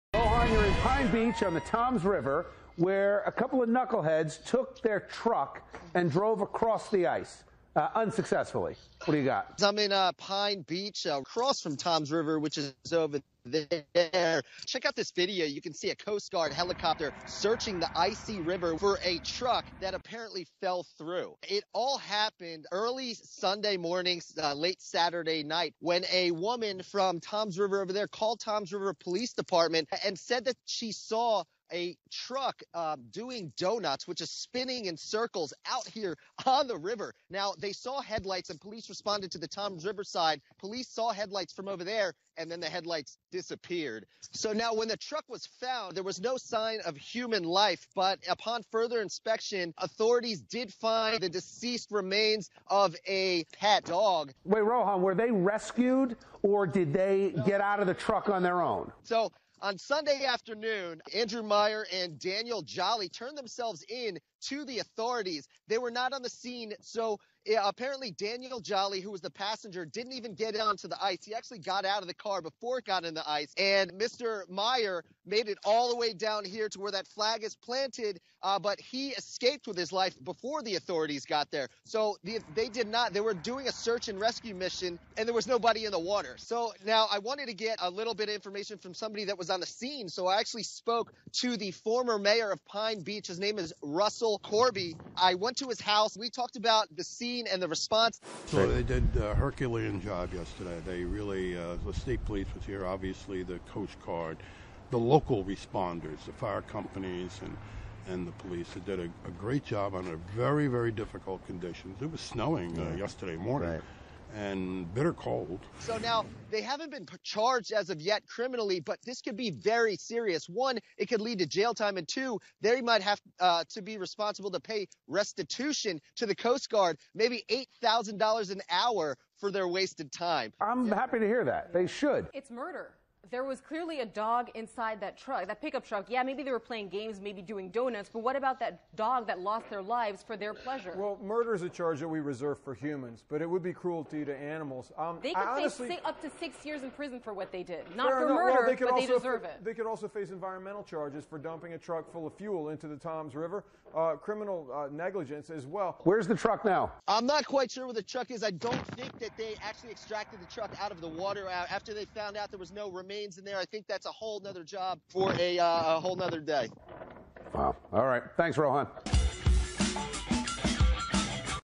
Truck falls through frozen river in Pine Beach
A truck drove into the frozen Toms River in southern New Jersey prompting a search from officials and discovered a pet dog dead with no one else inside.